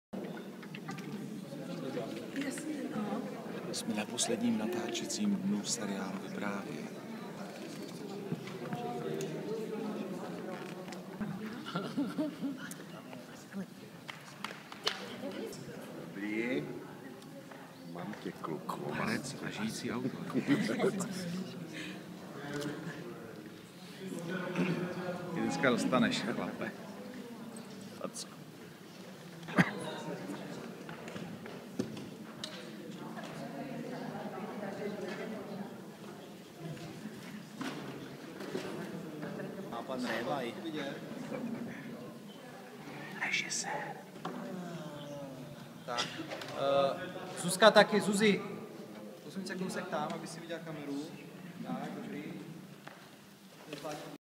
Posledni klapka Vypravej
Dnes padla na zámku v Dobříši posledni klapka seriálu Vyprávej. Malinko jsem zamáčkl slzu. Pět let je pět let ;(